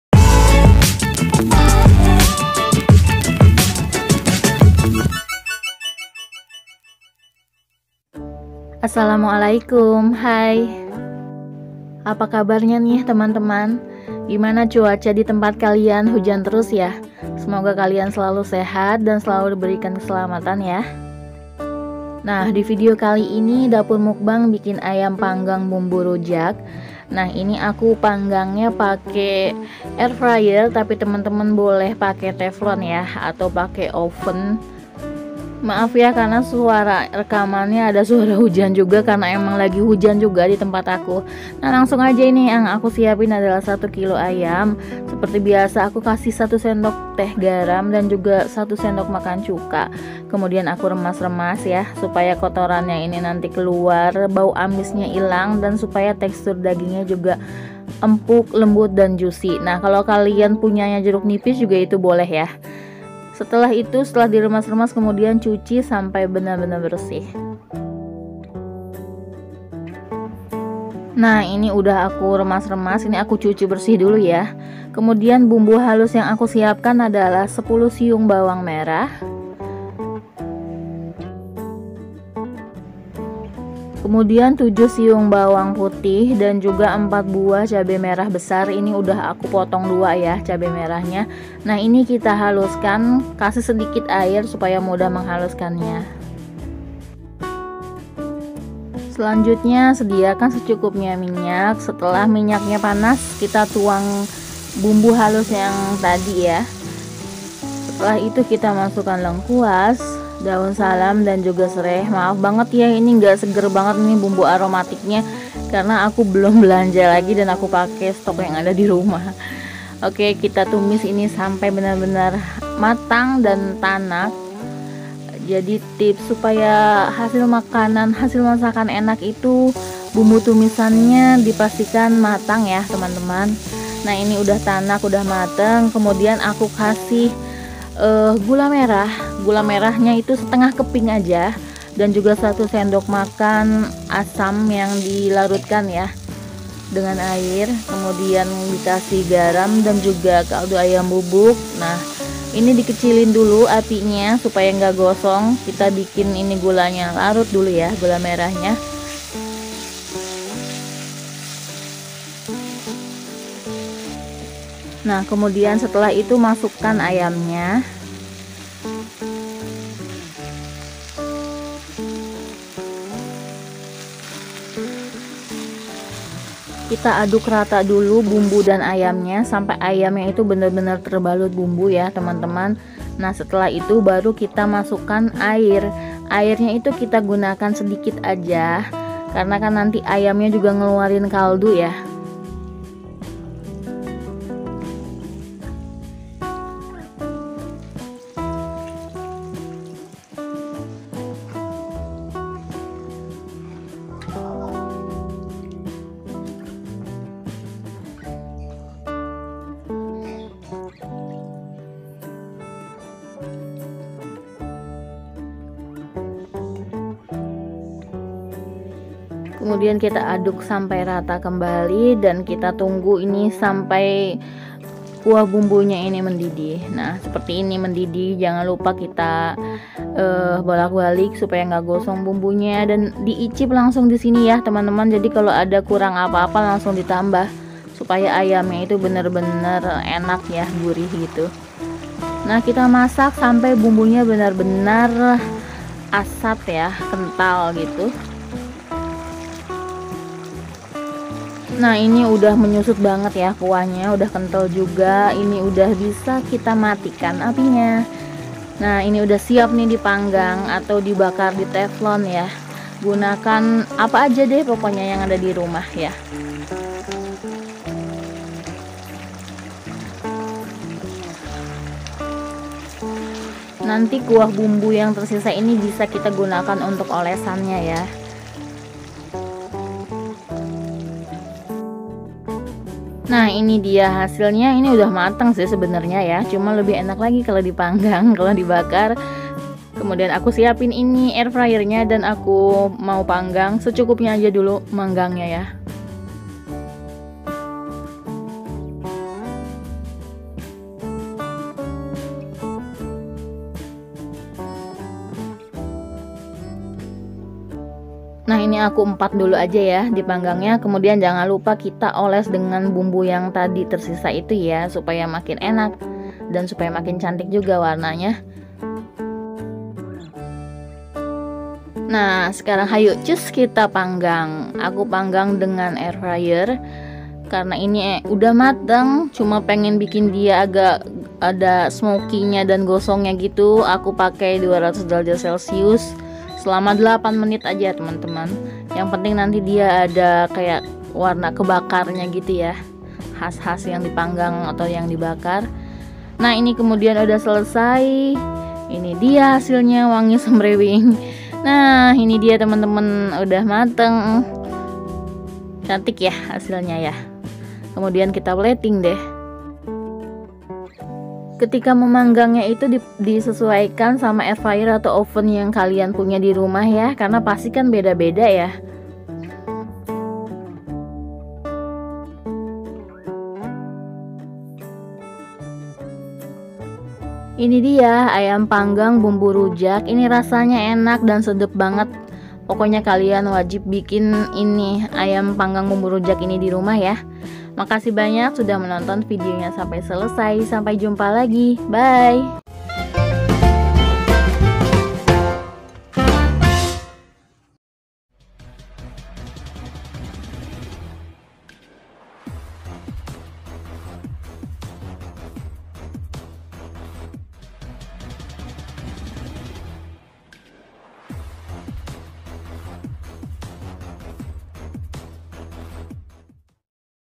Ayam Panggang terenak kalian wajib recook ni bund 😋!! Resep Ayam Panggang/Bakar Bumbu Rujak
ayam Panggang / bakar bumbu rujak

* 1 kg ayam
* 10 siung bawang merah
* 7 siung bawang putih
* 4 buah cabe merah besar
* 1/2 keping gula merah (kecil)
* 1 sdm asam Jawa ( 1 bungkus kecil larutkan air)
* garam dan kaldu ayam secukupnya
* secukupnya air
* lengkuas, daun salam dan sereh

Untuk cara memasak nya silahkan tonton videonya sampai selesai ya bund🥰
Terima kasih😘💕